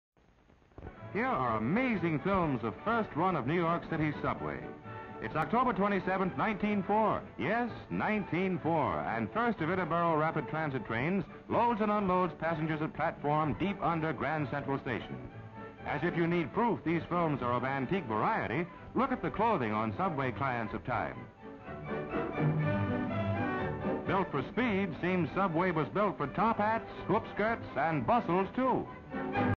October 27 1904 First Run of New York City Subway
CZ NYC Subway footage, circa 1904

First run of New York City's subway, October 27, 1904 FIRST INTERBORO RAPID TRAIN in Grand Central Station  showing pedestrians on platform wearing top hats, bustles, and hoop skirts.